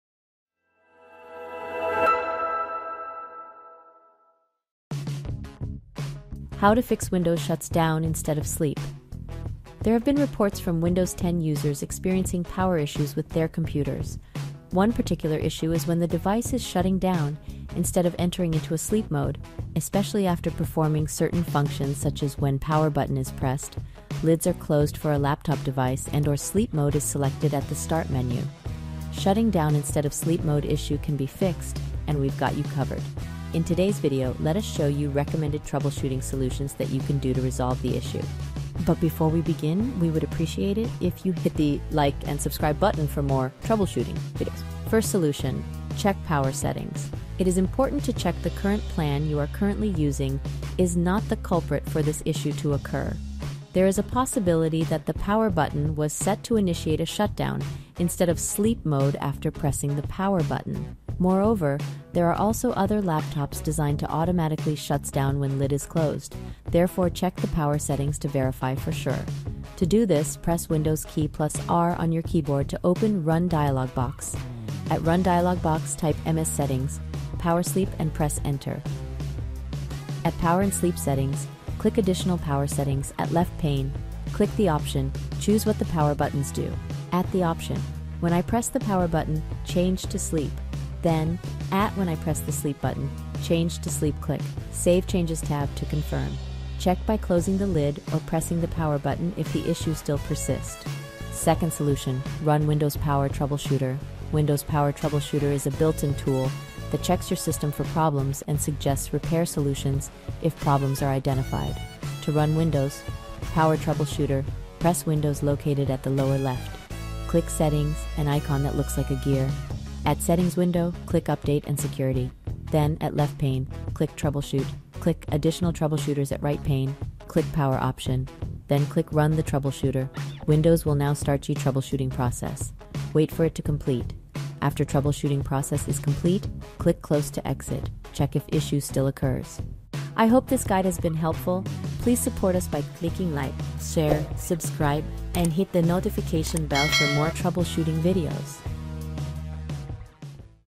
How To Fix Windows Shuts Down Instead Of Sleep
Shutting down instead of entering into sleep mode on your computer is a very frustrating experience. If you are experiencing this kind of problem, we've got you covered.

For today's video. we have listed two troubleshooting solutions that you can do to fix the problem. After this video, you will now be able to restore your computer's sleep function according to your own preferences.

00:00 - 00:38 Introduction
00:45 - 01:40 Check Power Settings
01:41 - 02:19 Run Windows Power Troubleshooter